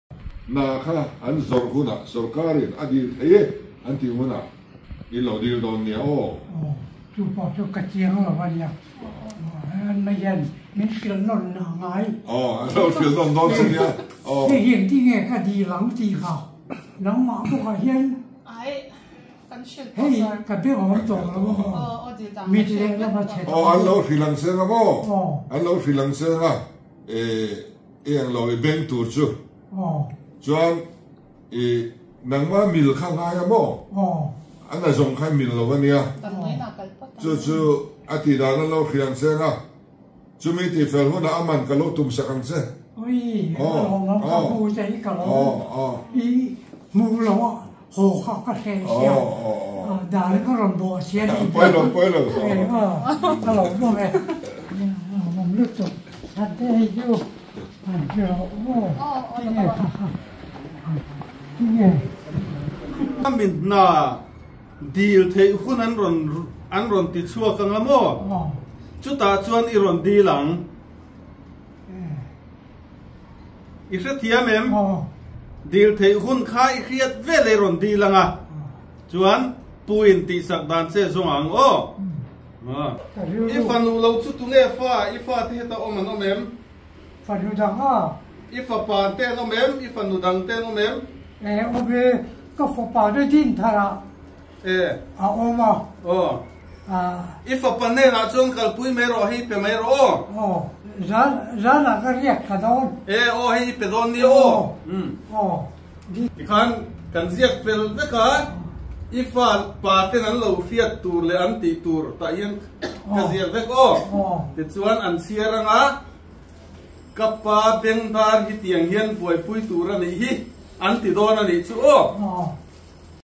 Humble & Kindness of Mizoram Chief Minister
One old man come to CM for his pending payment issue. CM allow to enter his cabin and listen his grievance.

Salute 🫡

Gis Suman Vlogs.